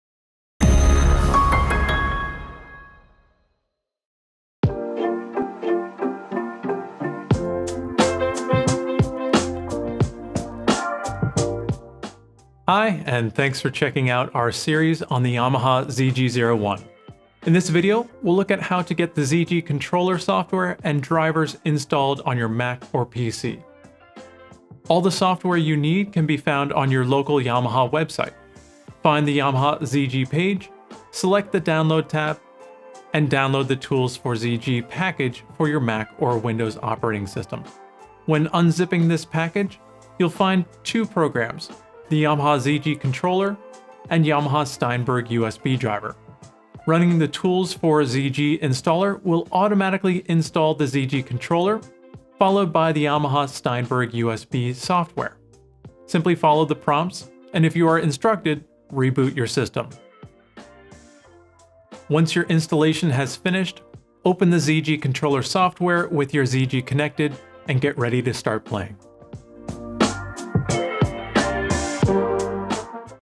Yamaha ZG01 Quick Setup Series: Installing TOOLS for ZG
In this video, we will look at how to get the Yamaha ZG Controller software and drivers installed on your Mac or PC.
* TOOLS for ZG will be available in early summer 2022.

The subtitles are available in the following languages:
English, 日本語

You can find out more about the ZG series.